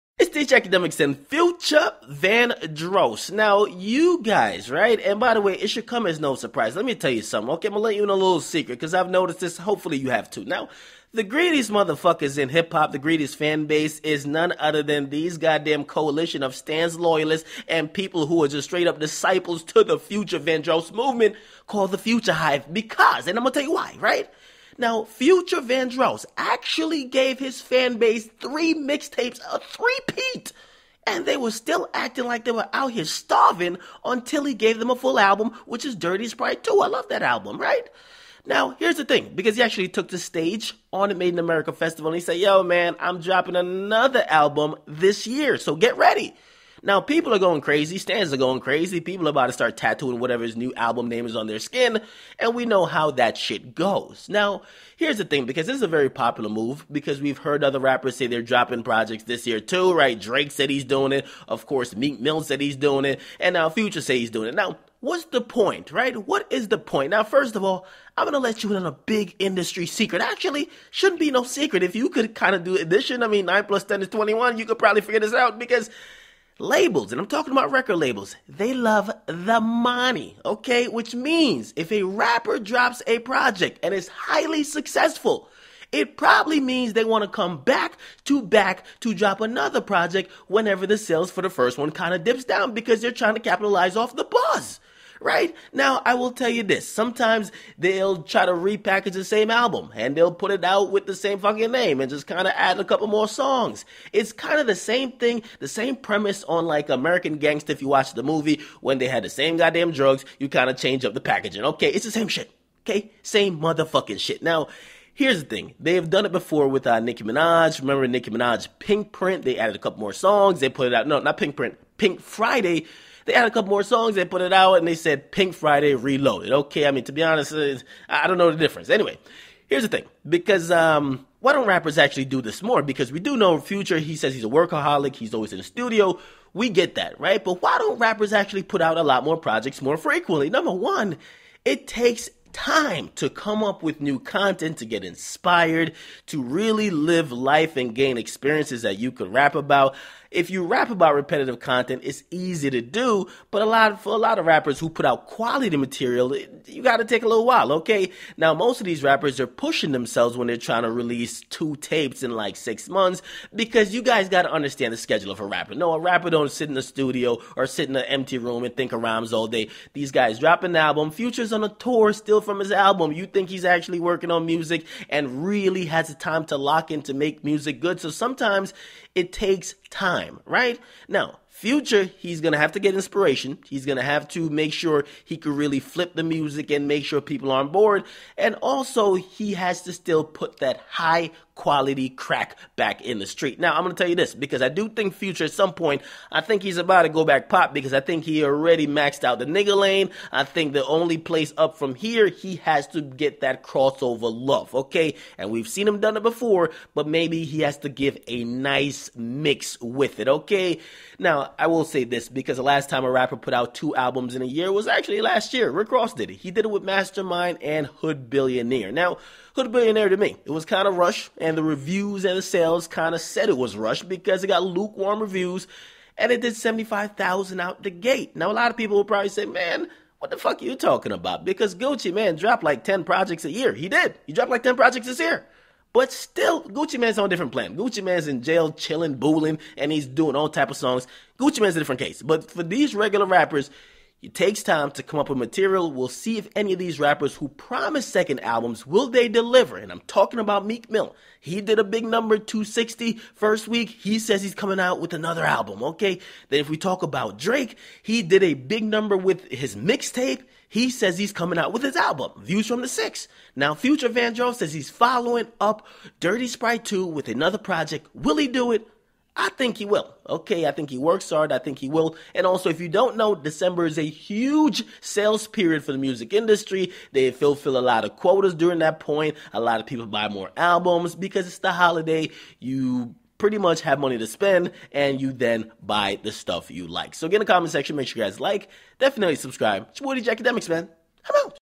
Future Announces That He Will drop a Second Album Before The Year Ends!
DJ Akademiks Speaks on Future Announcing That He Will drop a Second Album Before The Year Ends!